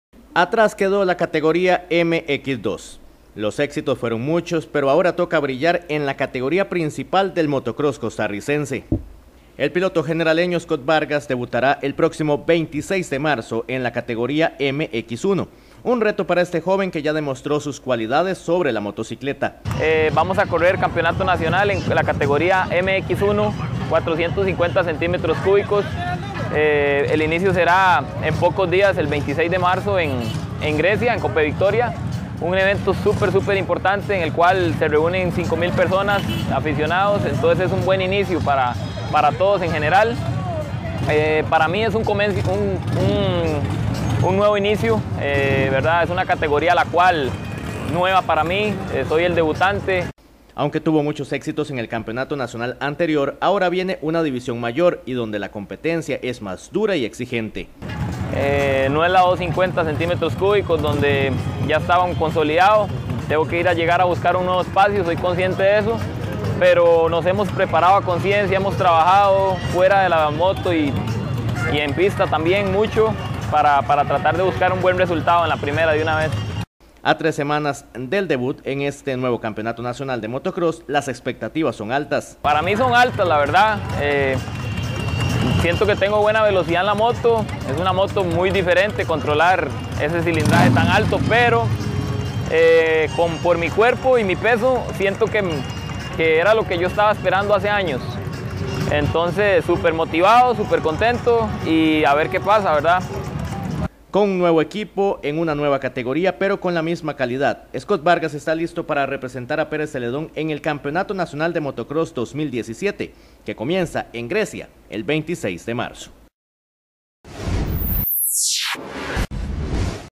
Scott Vargas debutará en la categoría principal del motocross el 26 de marzo de 2017
Atrás quedó la categoría Mx2, los éxitos fueron muchos, pero ahora toca brillar en la categoría principal del motocross costarricense.